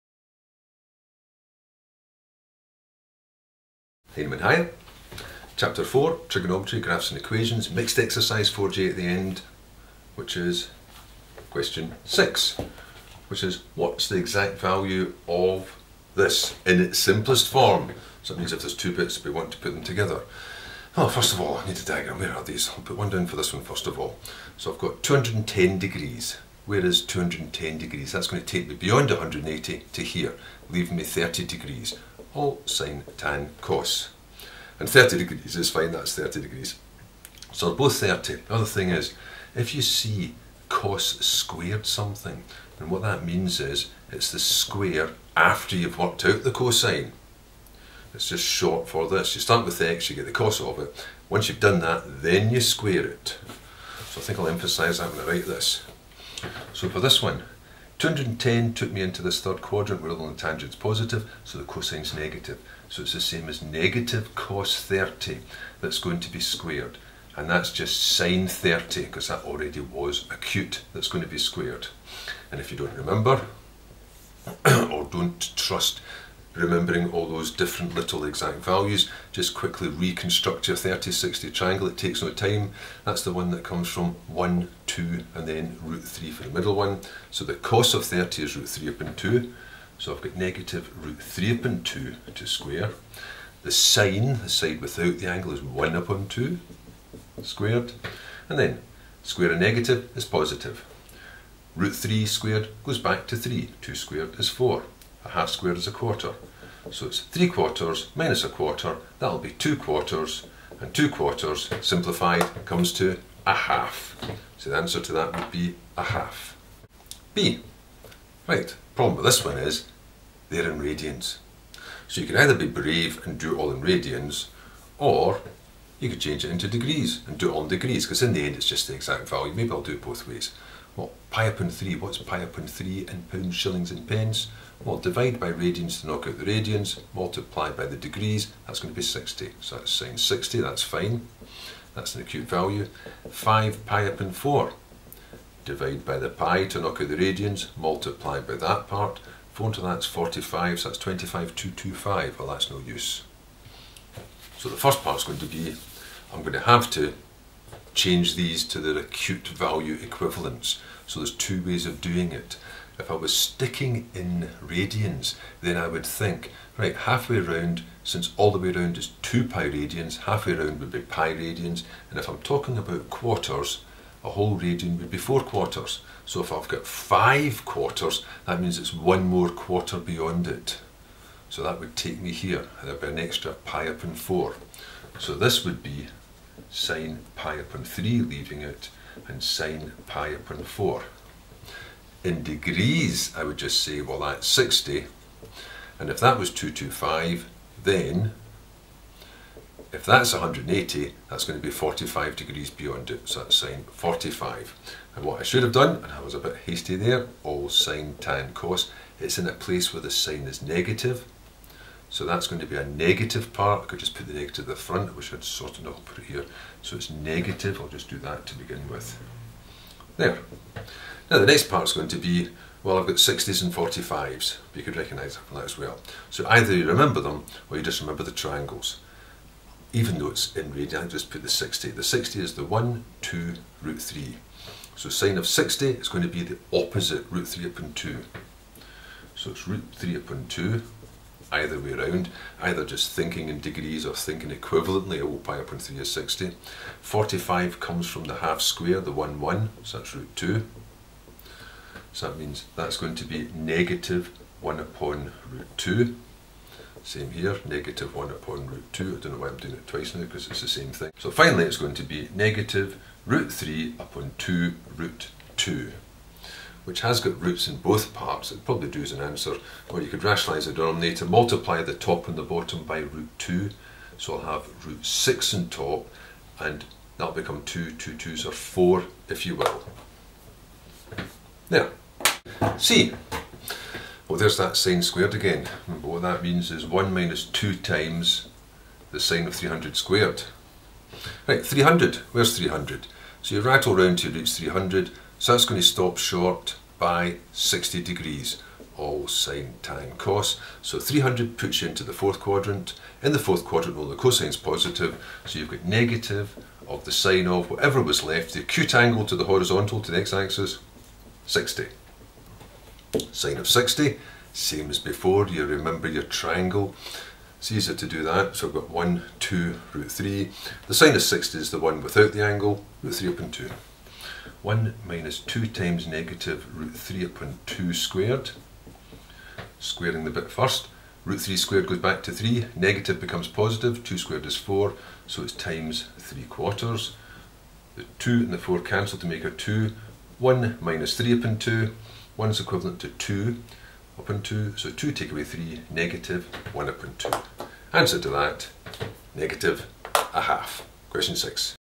Higher Revision: Ex 4J no.6 (Heinemann)
Unit 1, chapter 4 -- Trigonometry: graphs and equations

Solution to question 6 of exercise 4J (p66).

Book required for the questions.